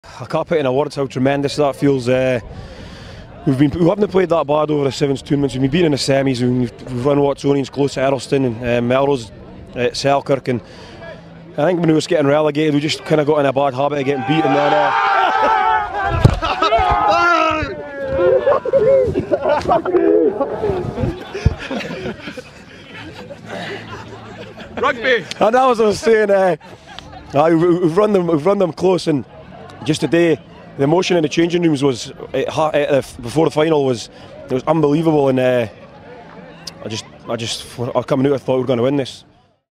RUGBY MAGIC MOMENT: ROSS GOODFELLOW OF JEDFOREST ATTACKED DURING INTERVIEW!
Ross Goodfellow ended his tremendous career by winning his own club's 7s tournament in 2013. Afterwards he announced his retirement but his team-mates were going to give him a send-off he wouldn't forget live on air!